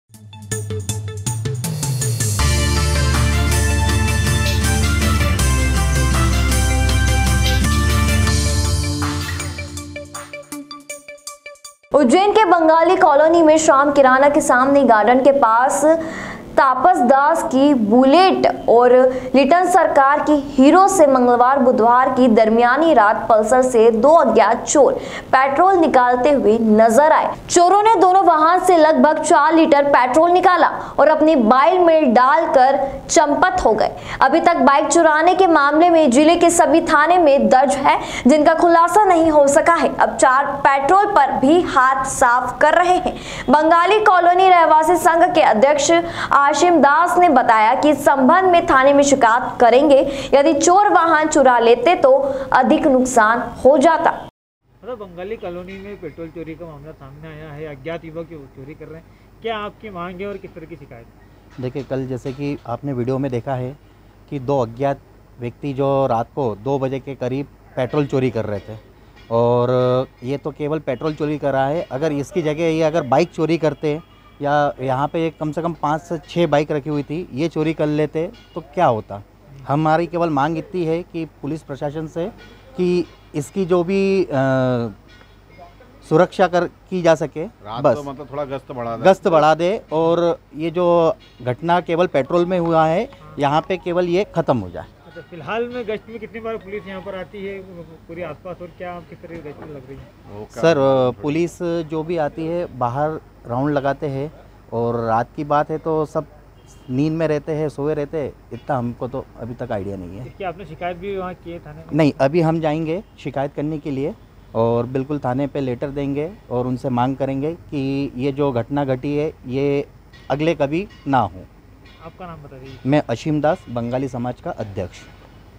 उज्जैन :- उज्जैन में चोरों का आतंक
बाइक से पेट्रोल निकालते हुए वीडियो आया सामने